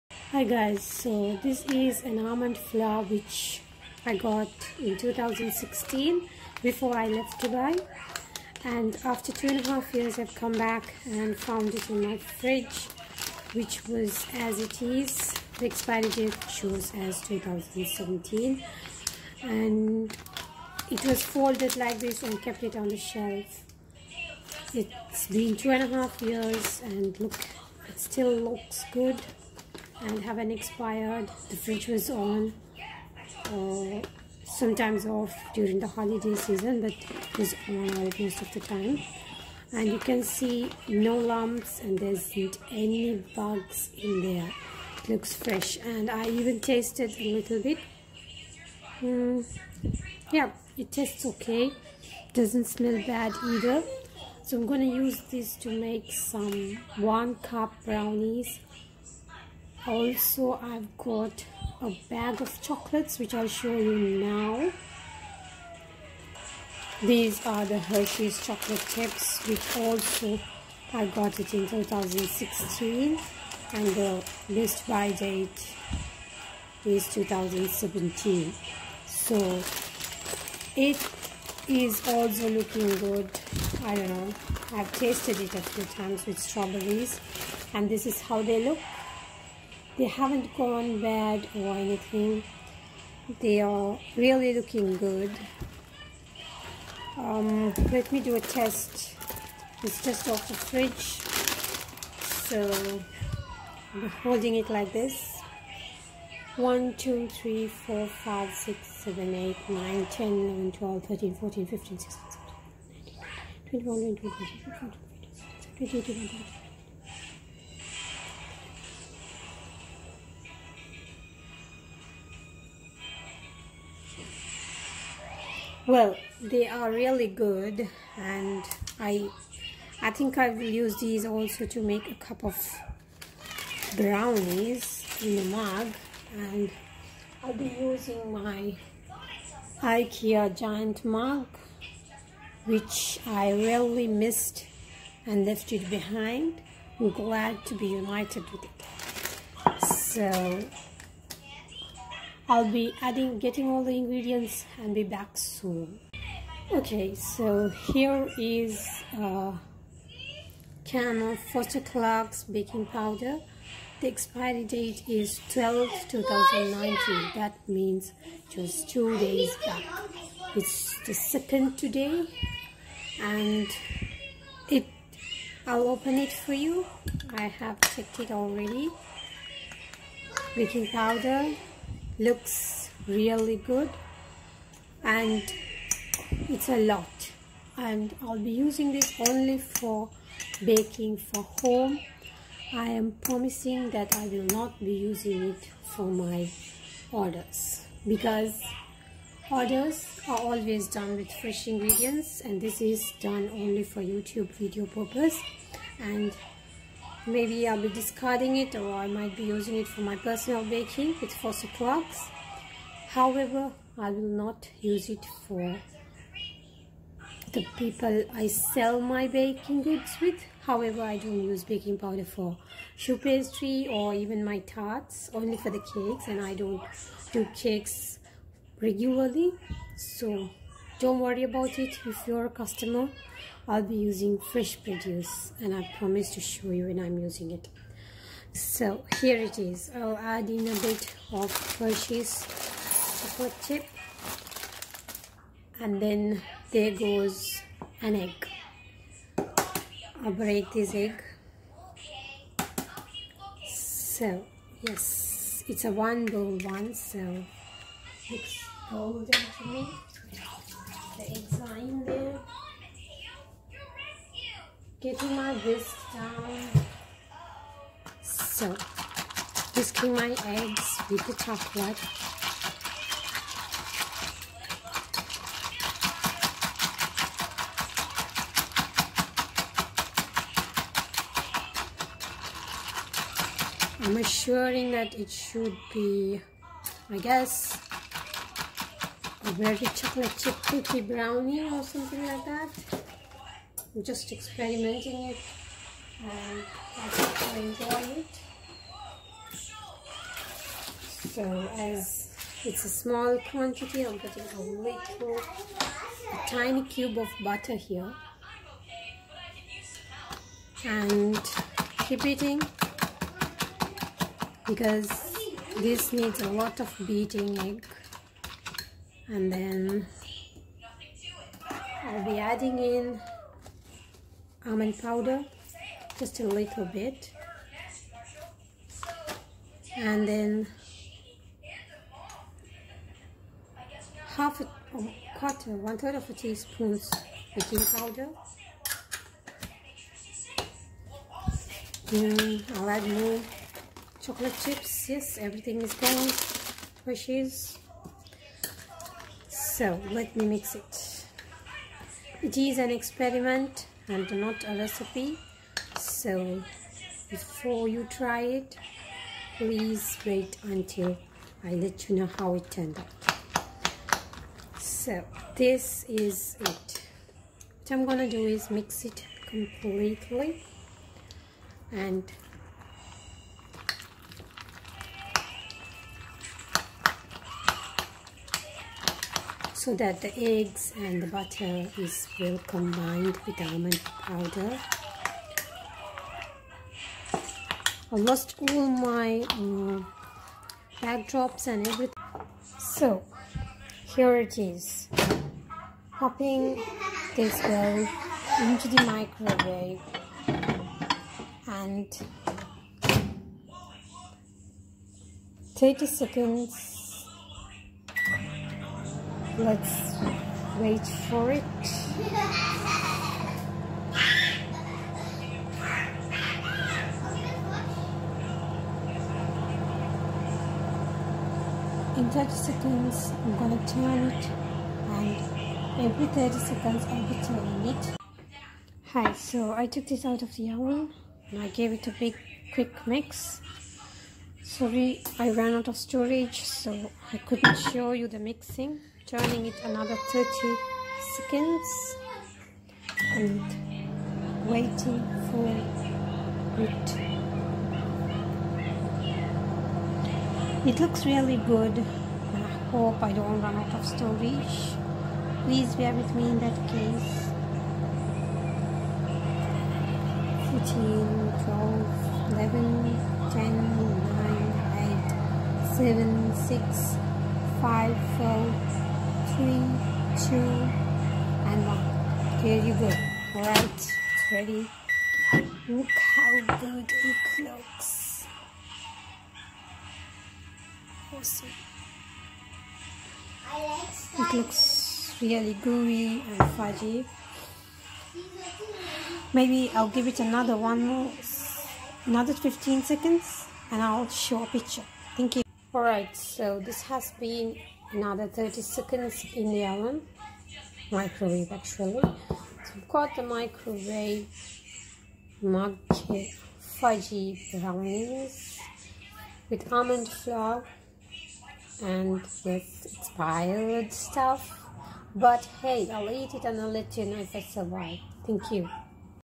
Worlds Weirdest Mug Brownie
I have created this video solely for fun and I ask you to not try it at home. Please note the taste of expired ingredients may vary so always make sure to taste test a small portion 24 hours before trying it out.
Also please note I did not feed this to my kids nor anyone. I ate it myself and felt normal.